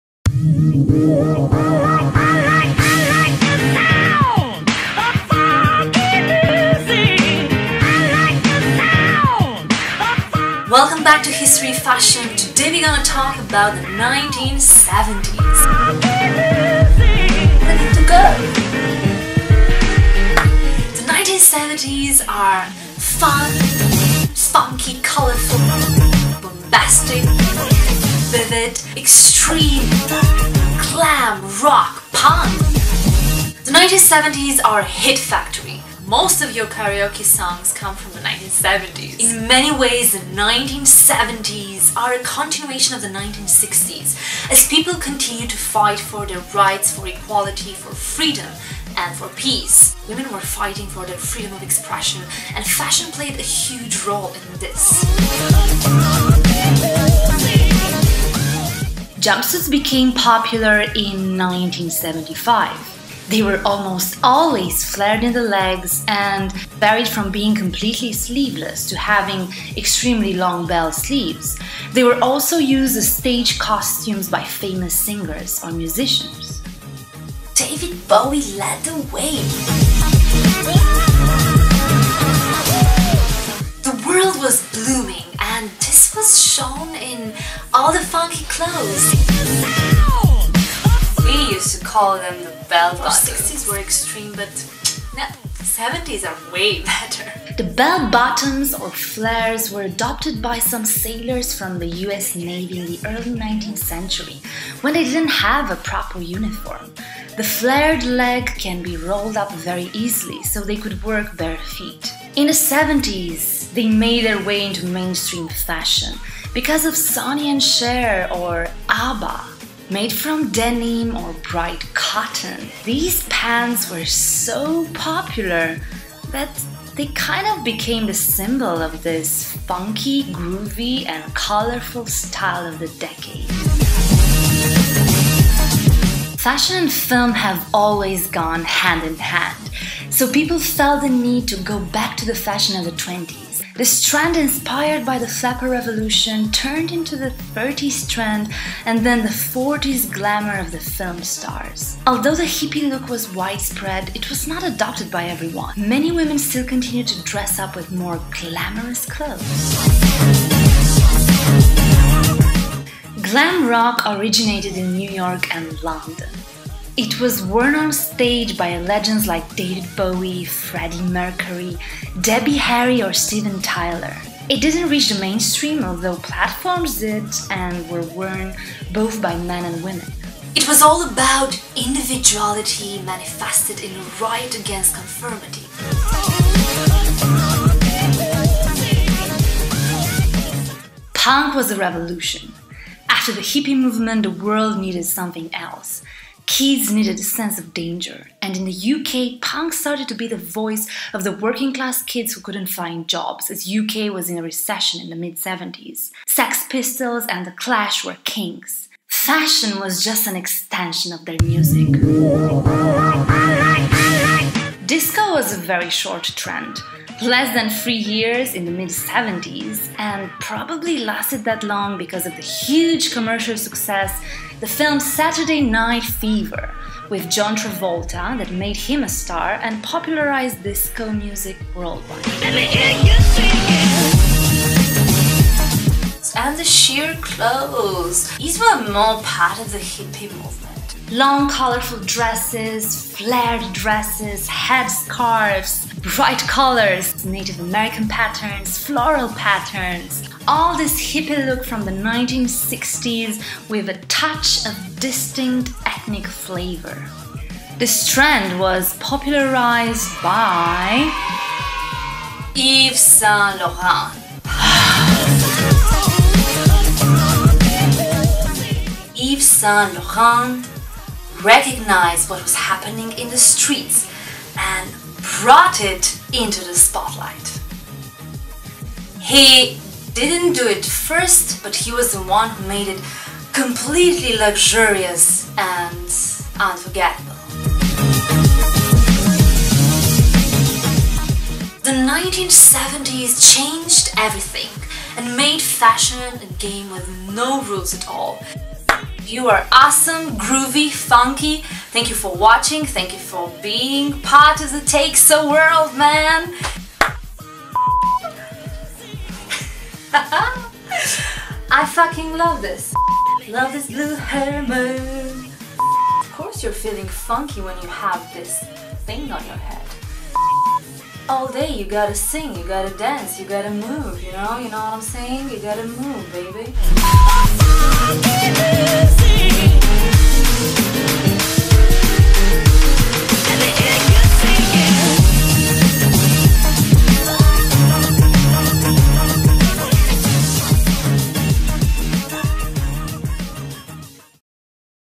Fashion of the 70s
The 1970s changed everything and made fashion a game with no rules at all! Everything was allowed from the mini skirts to the platform shoes, satin pants and all the glam and glitter. This era was the birth of “trends”. And there were so many trends as women started to wear whatever they wanted from the glamorous styles of the 40s to flared pants, punk styles or long colorful dresses.

The outfit at the beginning of the video is from the new collection VALENTINE by TEYXO!